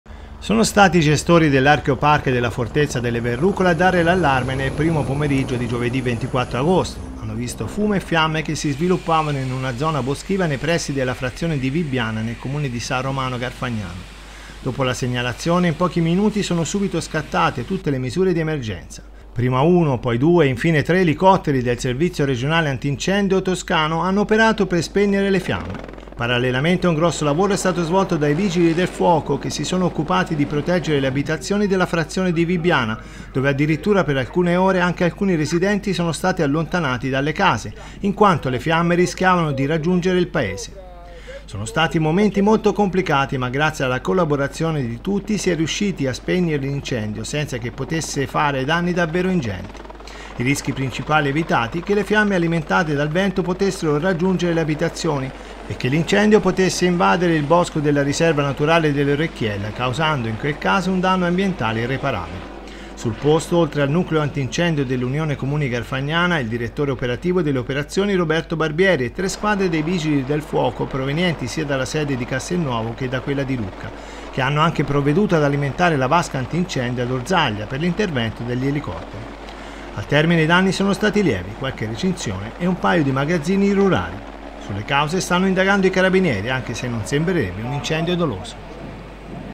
Incendio a Vibbiana, situazione sotto controllo
Sono stati momenti molto complicati ma grazie alla collaborazione di tutti si è potuto spegnere l’incendio senza che potesse fare danni davvero ingenti; i rischi principali evitati che le fiamme alimentate dal vento potessero raggiungere  le abitazioni e che l’incendio poetesse invadere  il bosco della riserva naturale dell’Orecchiella causando in quel caso un danno ambientale irreparabile.